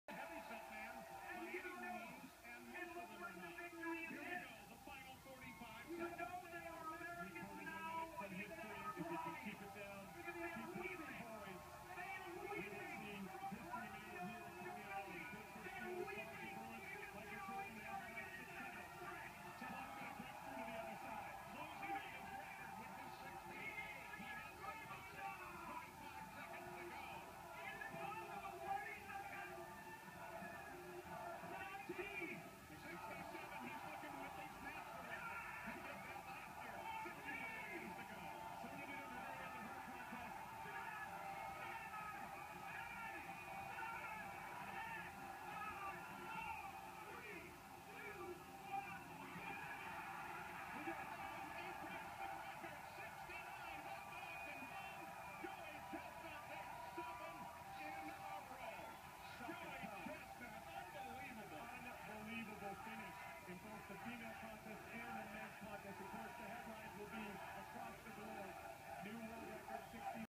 Joey Chestnut eats 69 hot dogs breaking world record
Joey "jaws" Chestnut breaks his own world record of 68 hot dogs by eating 69 in 10 minutes.